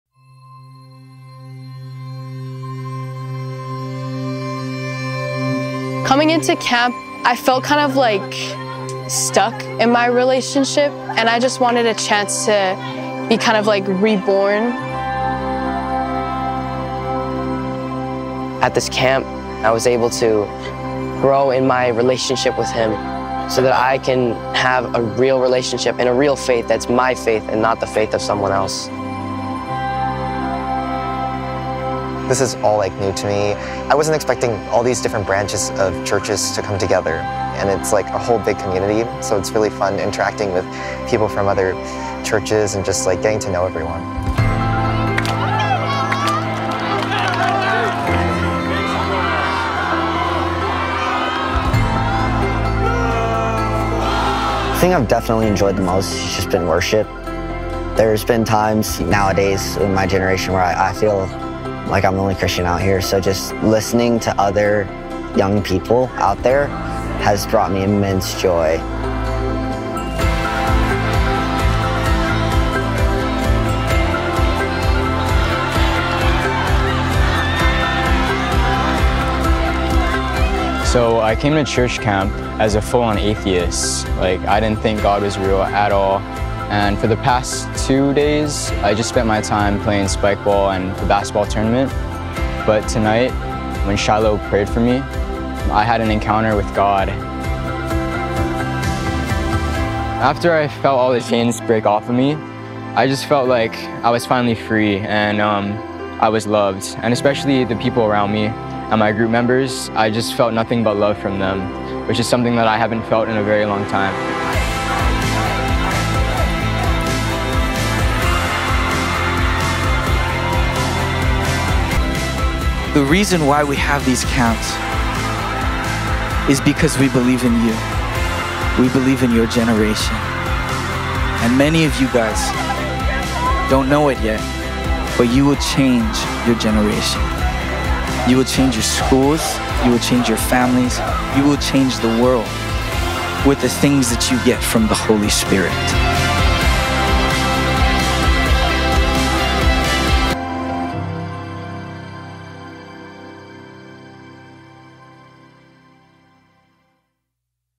Camp Kakou High School HCM 2023
Checkout highlights from Youth Camp 2023 High School

Some testimonies include:

Students set free from suicidal thoughts and orphan spirit/mindset
Complete physical healings of backs, ankles, etc.
Students hearing God’s voice for the first time
Students making first-time decisions to follow Jesus
Jr. High & High School student-led worship teams
Students solidifying their identities as sons and daughters
Students coming in as atheists and now following Jesus